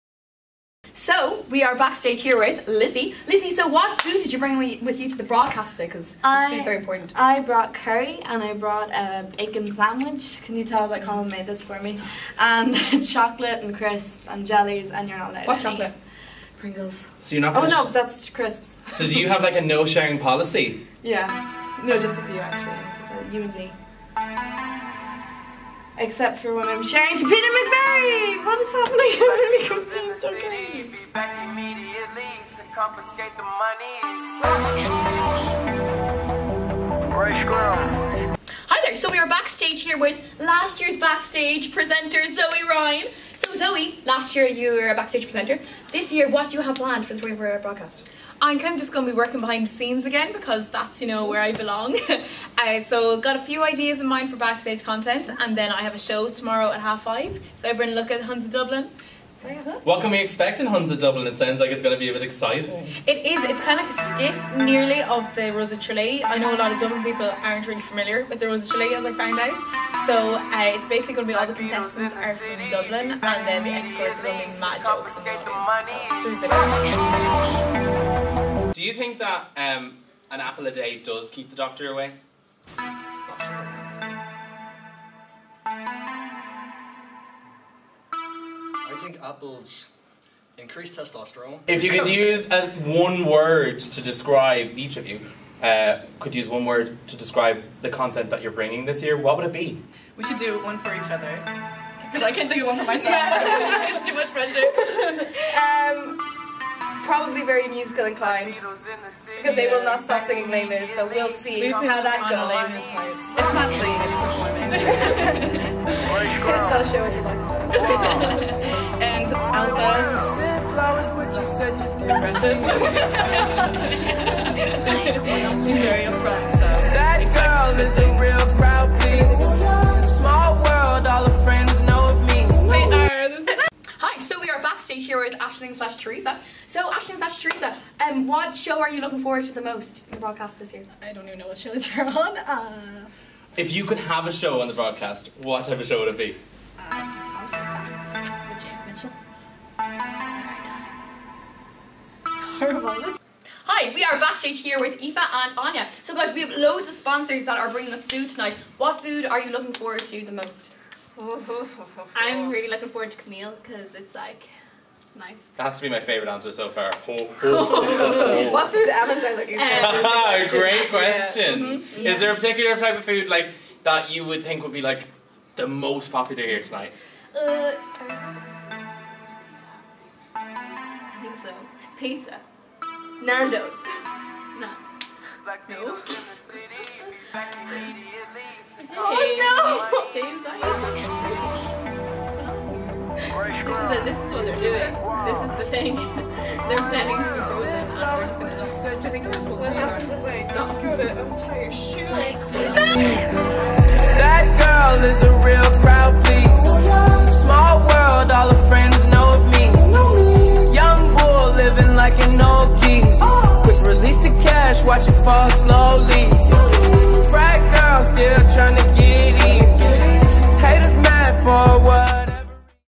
DCUtv24 | Mannequin-Interviews
Our DCUtv 24-Hour Broadcast 2016 Backstage Presenters, James & Caoimhe, take inspiration from the latest Internet-craze, 'The Mannequin Challenge', and conduct some 'Mannequin Interviews' around Broadcast-Land, a.k.a. The Henry Grattan Building in DCU.

make sure you keep donating to The Peter McVerry Trust, the link is on our website, and make sure you keep using DCUtv24.

Love James & Caoimhe? Love MPS!